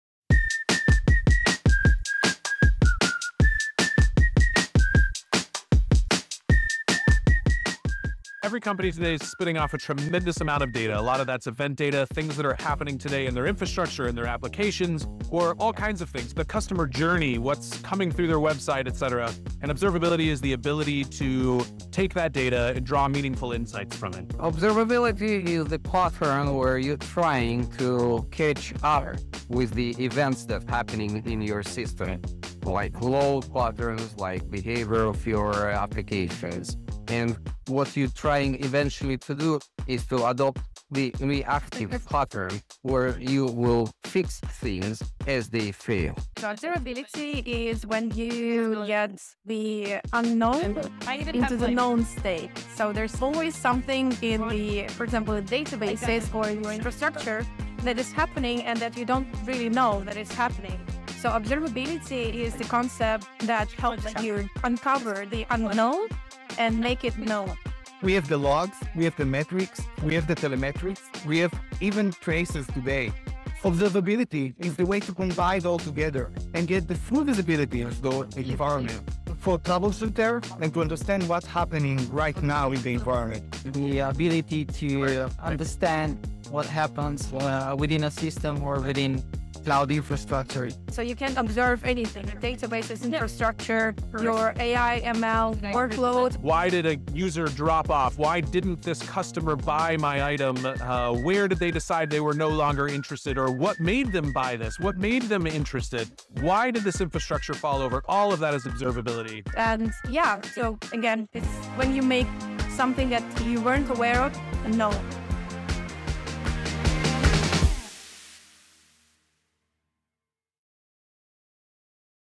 What is Observability? | Observability Explained by Experts
Observability is a concept within software development that allows you to understand what's happening inside complex systems. It's like having X-ray vision for your system or a water filter for your data, giving you the ability to see how it's performing, identify issues quickly, and ensure smooth operation.

This video features explanations of Observability from five experts. They discuss benefits like:
Understanding System Health - Learn what metrics, logs, and traces tell you about your system's inner workings.
Troubleshooting Made Easy - Pinpoint problems faster by correlating data from different sources.
Proactive Monitoring - Catch issues before they impact users and prevent downtime.

Whether you're a developer, ops professional, or just curious about keeping your systems healthy, this video is the perfect intro to Observability!

Special thanks to the experts who helped explain Observability:
Kendall Miller of Axiom
Eli Birger of @perfectscale_K8s
Sasha Mitroshkina of @percona
Ofir Stein of Apono
Nick Bordea of CloudGeometry

Monitoring vs. Observability: What’s the Difference?